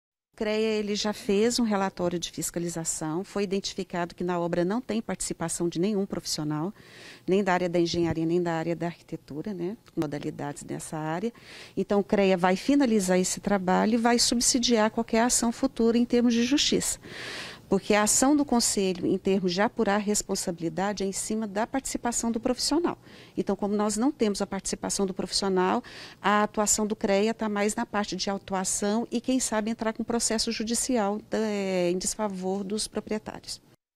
OBRA IRREGULAR: TETO DE GALPÃO DESABA E MATA TRABALHADOR
UM LEVANTAMENTO FEITO PELO CREA, MOSTRA QUE A OBRA NO GALPÃO ONDE FUNCIONAVA UMA LOJA DE MÓVEIS NO JARDIM GUANABARA ERA IRREGULAR. A POLÍCIA CIVIL VAI INVESTIGAR A FALTA DE PROFISSIONAIS PARA ACOMPANHAR A OBRA E, UM NOVA PERÍCIA FOI PEDIDA NO LOCAL DO DESABAMENTO.